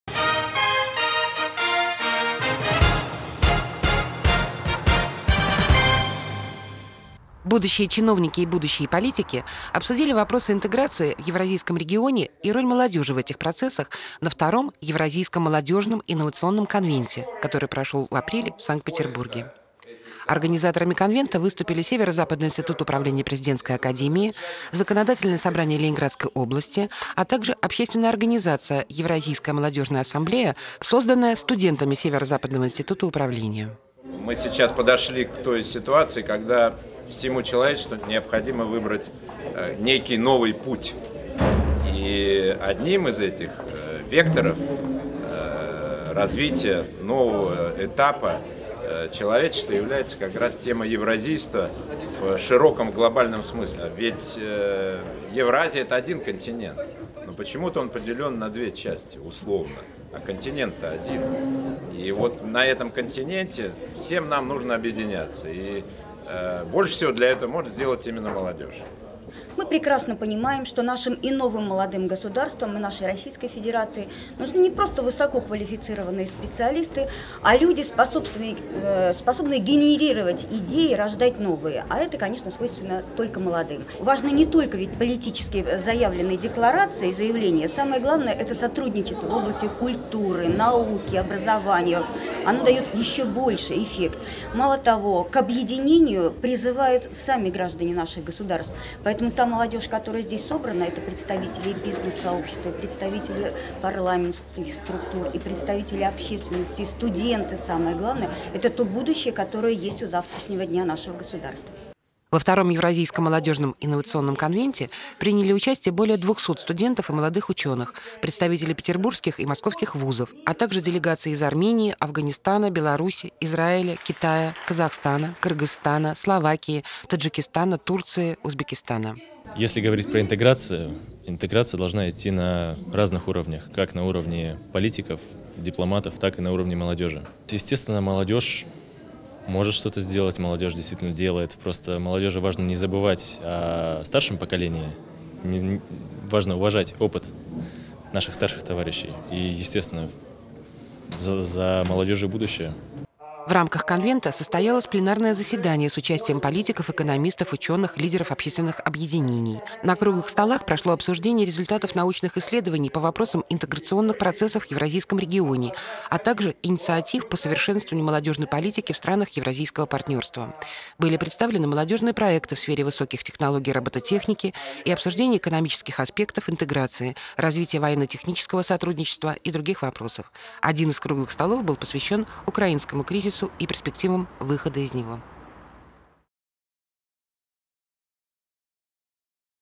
Второй евразийский молодежный инновационный конвент
С 8 по 10 апреля 2015 года в Санкт-Петербурге прошел второй евразийский молодежный инновационный конвент, организатором которого выступил Северо-Западный институт управления РАНХиГС